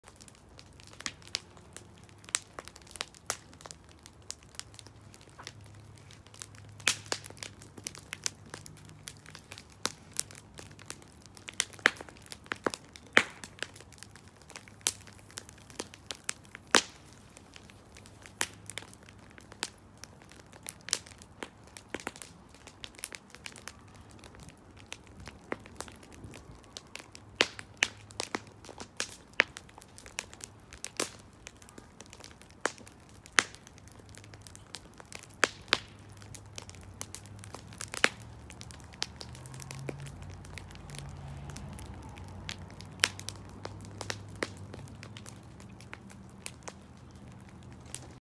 Eating popsicle and listening to the fire 🔥
I love Down syndrome Ivylee